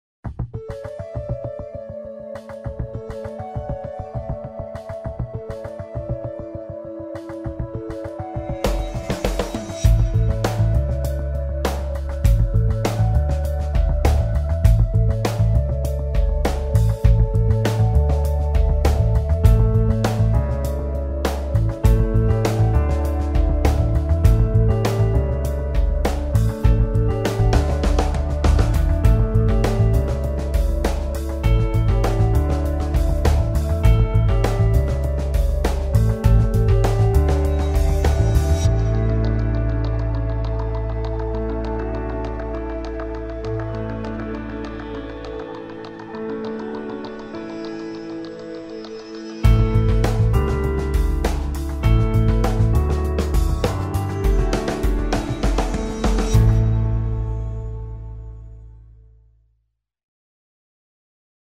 Unleash The Entrepreneurial Potential of Your Organization
The message was to "Learn to Unleash the Entrepreneurial Potential of Your Organization". Produced by Viva Productions.